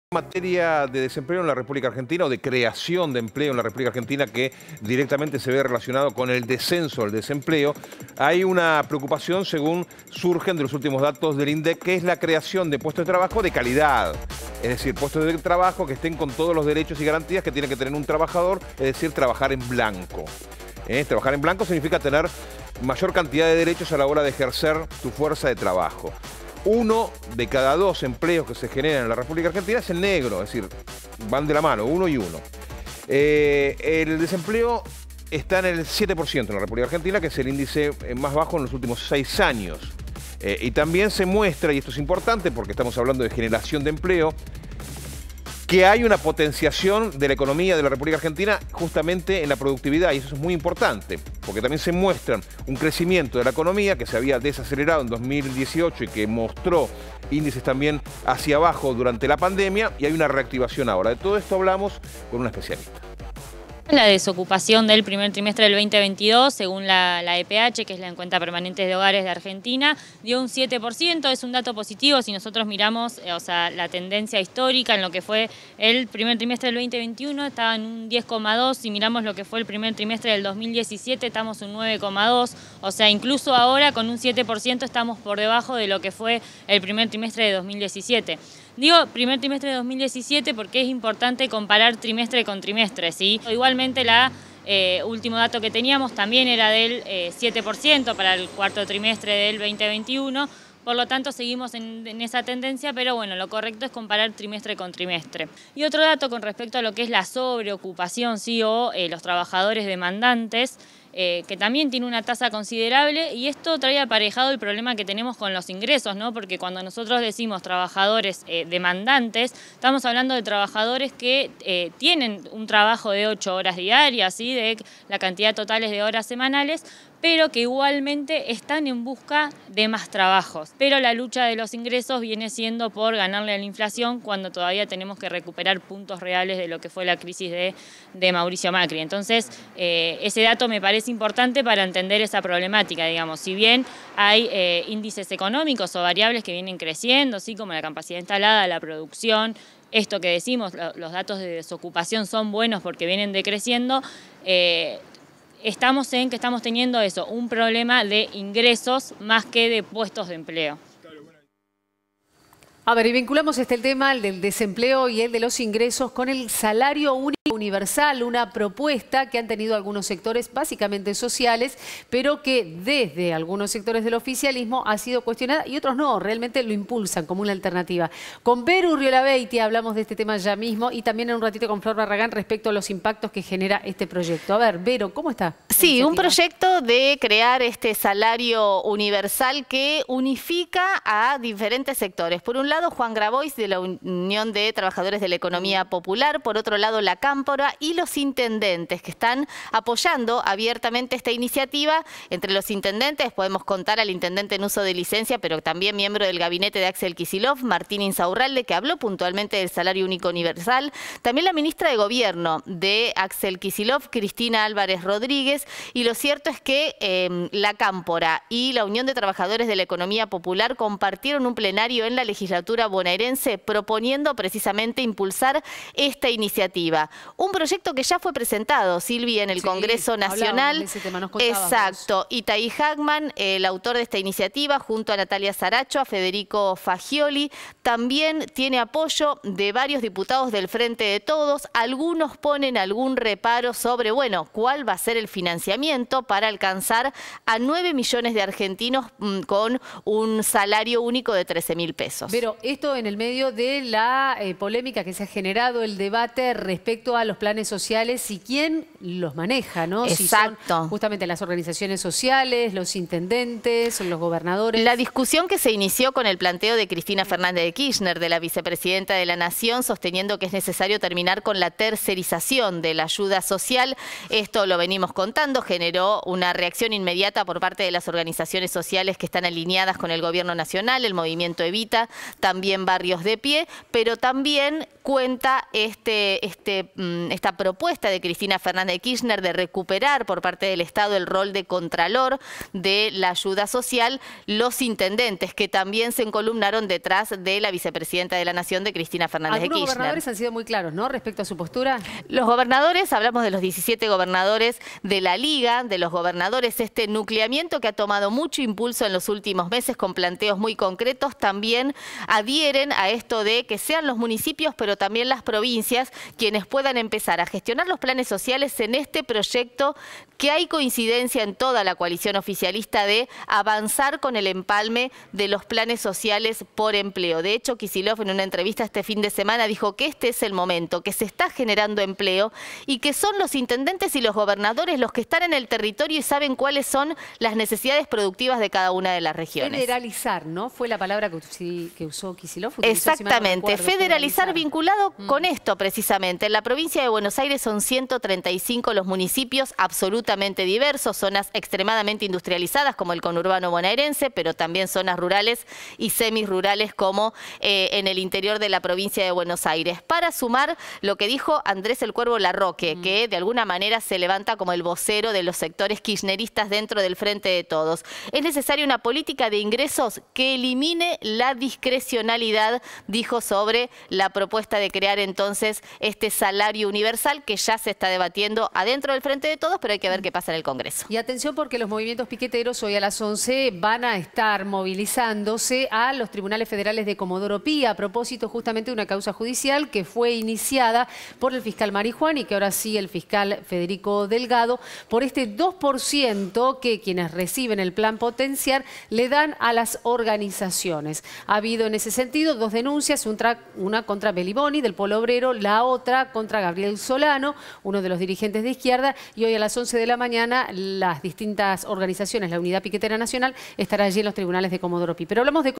El nivel del desempleo y el ingreso para todos
La economista del CEPA Luciana Bilbao analiza las últimas cifras de la desocupación que se mantiene en los niveles más bajos de los últimos seis años. Por otro lado, Verónica Urriolabeitia comenta el proyecto del salario único universal y el empalme de planes por empleo.